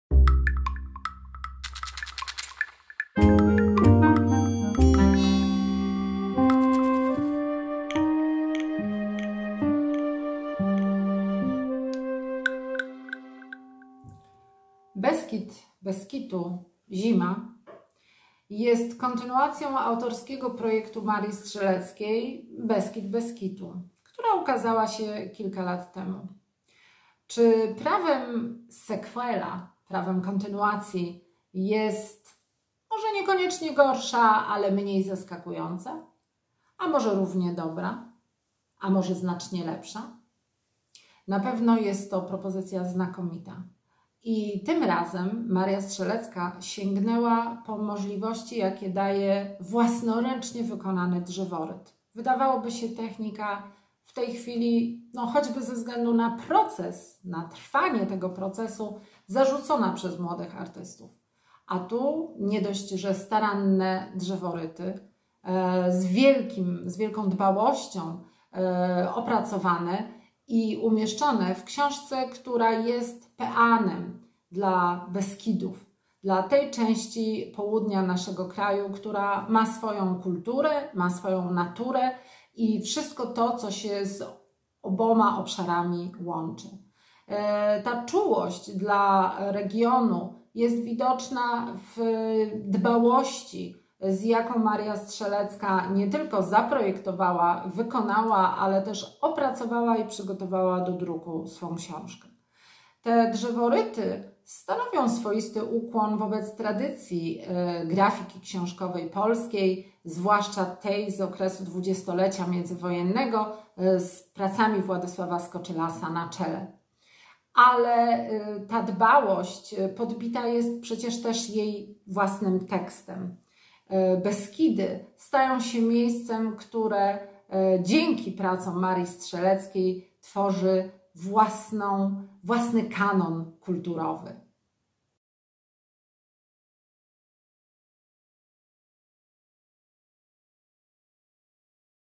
"Beskid bez kitu. Zima" Nominacja graficzna w konkursie Książka Roku 2022 PS IBBY
O książce „Beskid bez kitu. Zima” Marii Strzeleckiej (Libra PL) nominowanej do nagrody graficznej w konkursie Książka Roku 2022 Polskiej Sekcji IBBY opowiada członkini jury, Anita Wincencjusz-Patyna.
Animacja: Ula Palusińska
Muzyka: Filip Dreger
Realizacja: Maciej Wróbel
Konkurs Książka Roku 2022 PS IBBY jest dofinansowany ze środków Urzędu Dzielnicy Mokotów m.st. Warszawy.